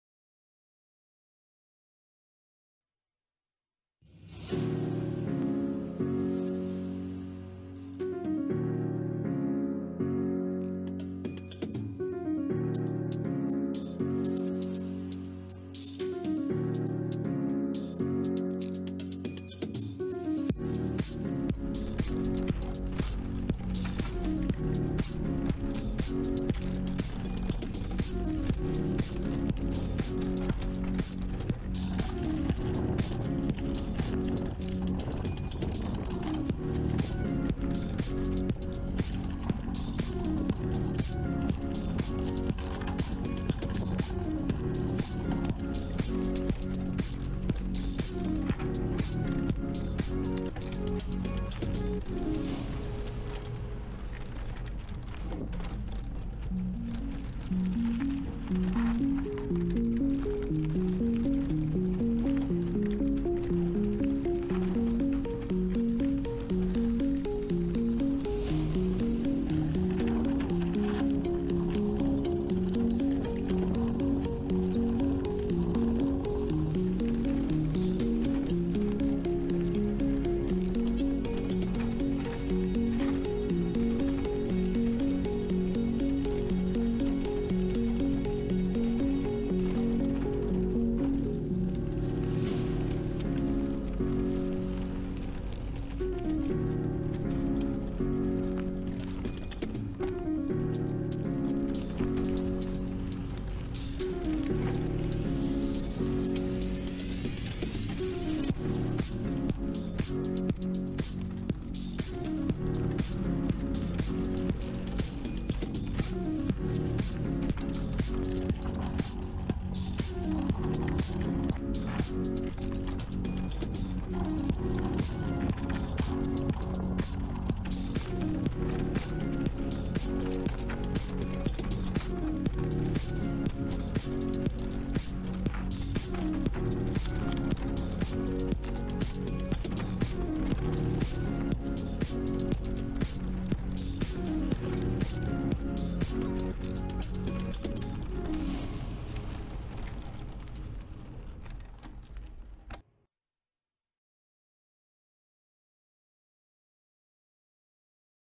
MTB - SNAKE
In questo video puoi seguire la discesa del sentiero mtb SNAKE dei Campiani, Brescia.